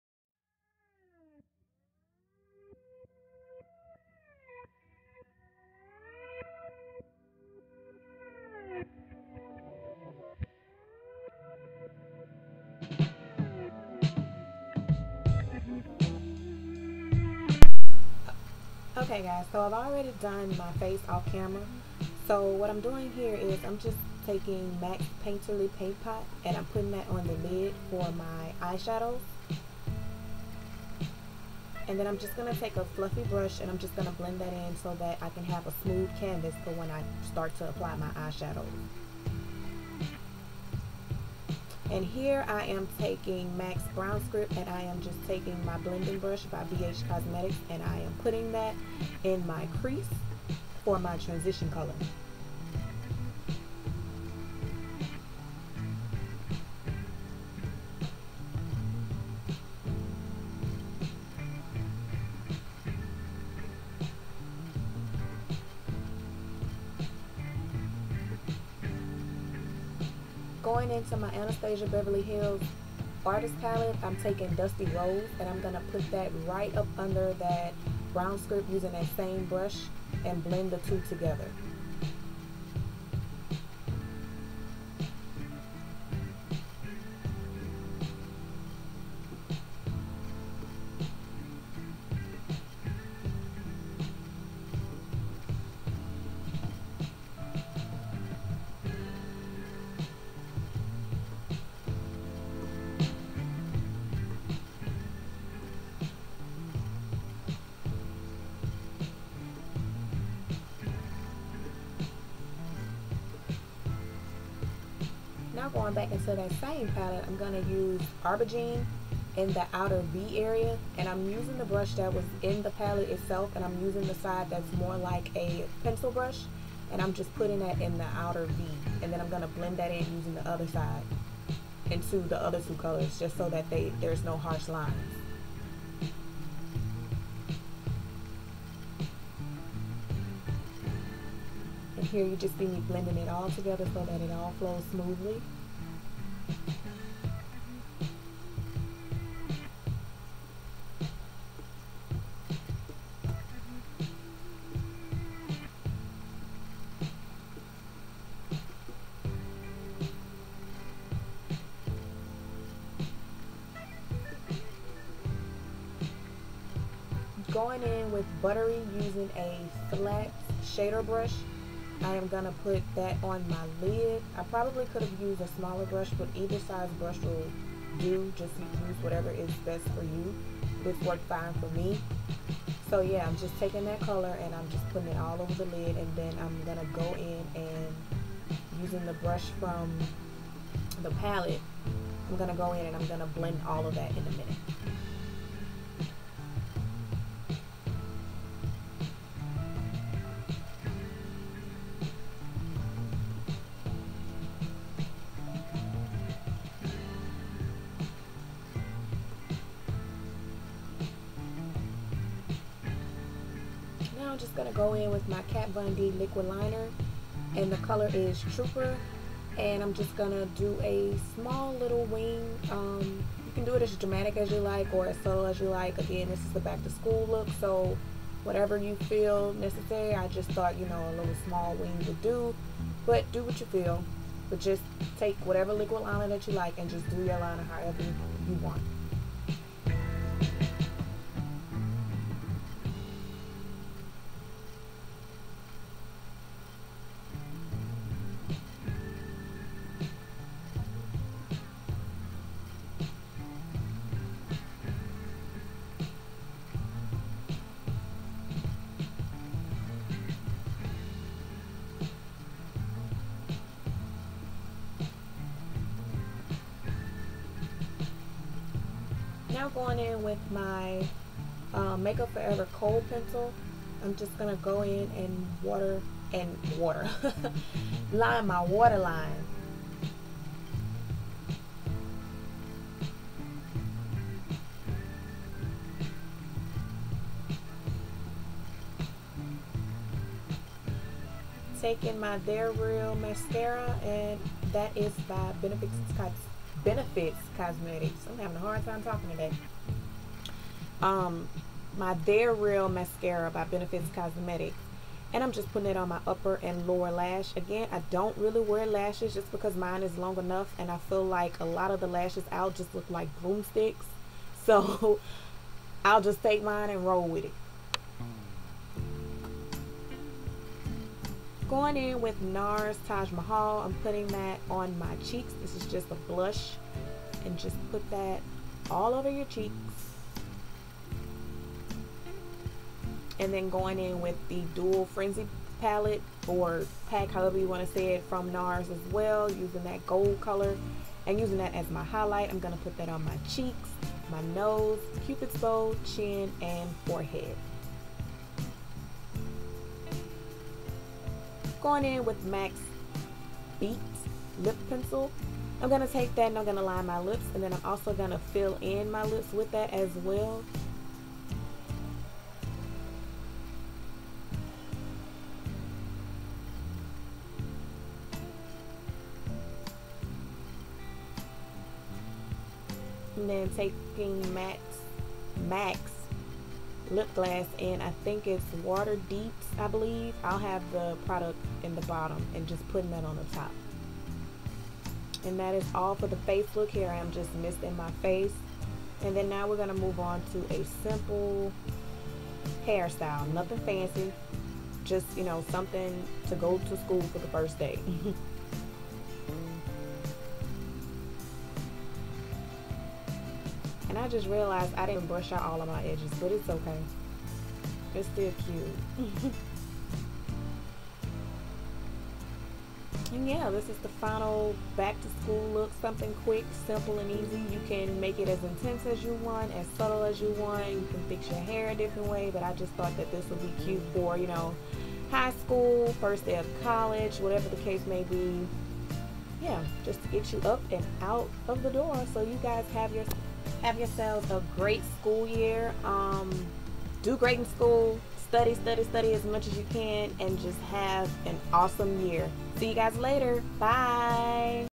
Back To School Simple Makeup Look 2015
Hi guys! Here is my back to school simple makeup look! I hope that it can be an inspiration to you guys if you are looking for a cute simple look for your first day back to school! Enjoy!